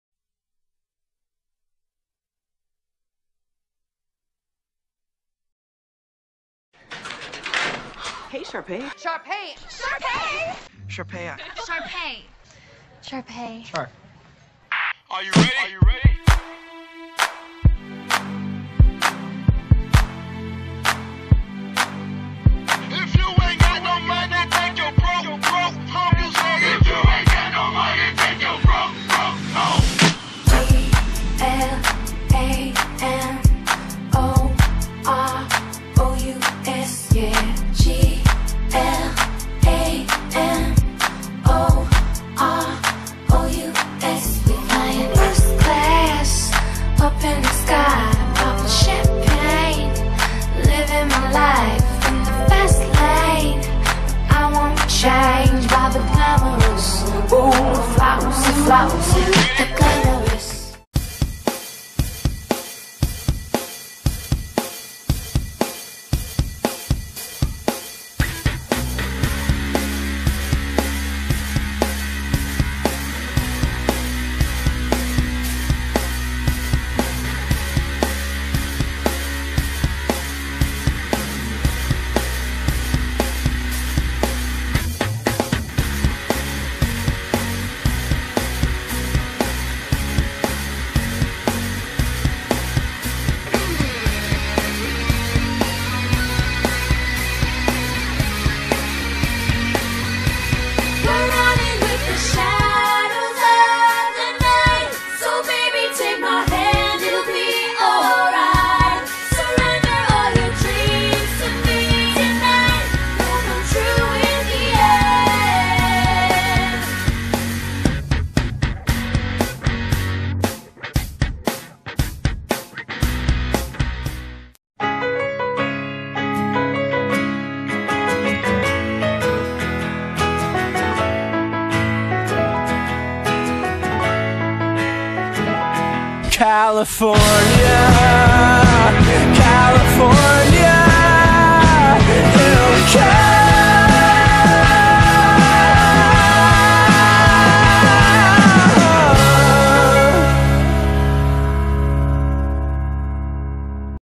Key Me In Trailer
Here is the trailer for a fictional spin off focusing on Sharpay's life after East High. It stars
Ashley Tisdale - Sharpay
Chris Lowell - Nick
Zachary Levi - Drew
Kaley Cuoco - Carmen

Synopsis: For one former wildcat, their journey takes a new direction. As Sharpay leaves the life at East High, she begins her new journey down at California. There, she'll meet new people and find her true self. Going after her dream may be a risk, but for Sharpay, it's a new journey in finding her own self through life.

Sadly, this is only a video and maybe you can use your imagination to create your own plot about it =)